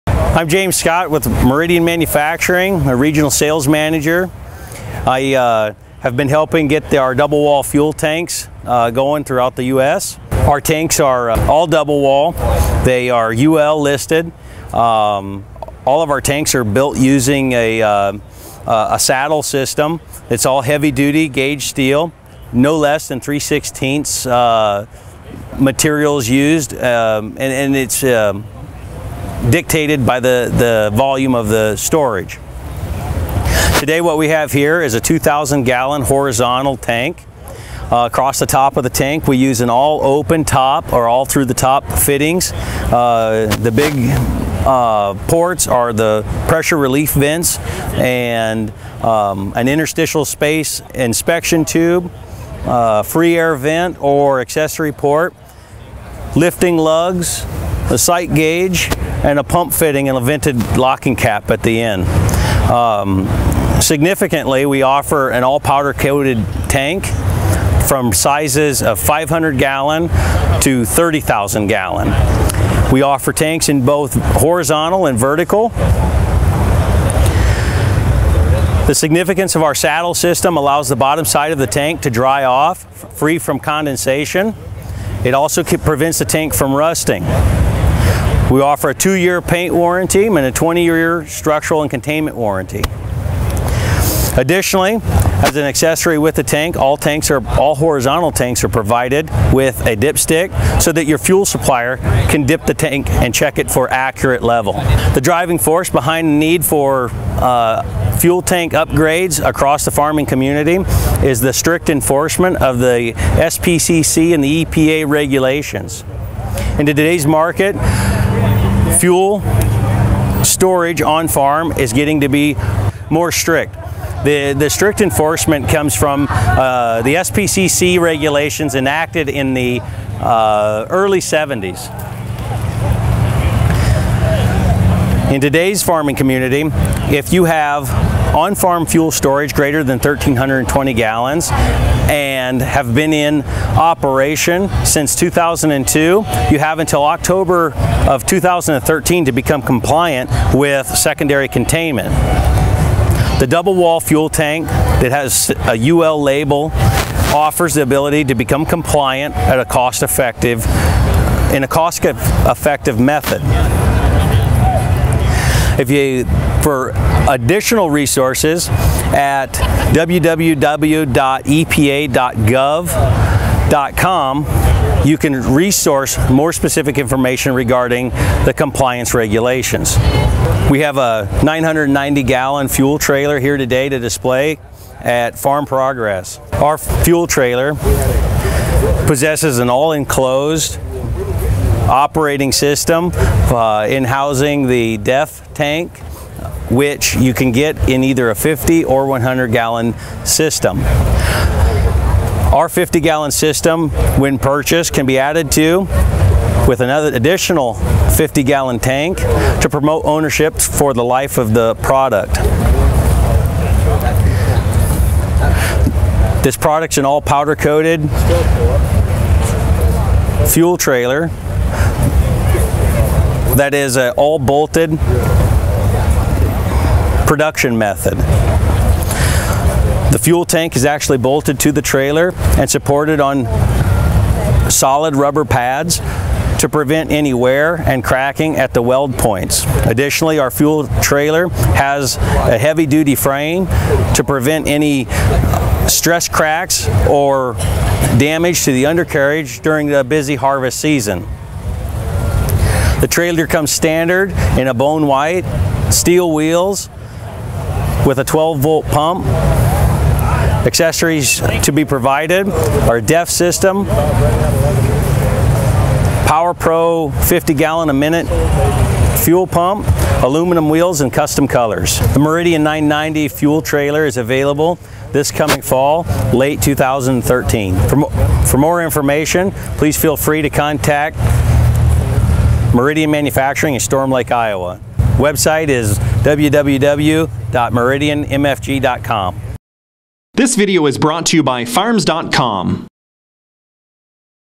Meridian Manufacturing's Double Walled Fuel Storage Tanks and 990 Fuel Trailer.
James from Meridian Manufacturing presents the  range of double walled fuel tanks from 500 gallon to 30,000 gallons both horizontal and vertical configurations.  He also introduces the 990 gallon Fuel Trailer that farmers can use during busy spring and fall seasons.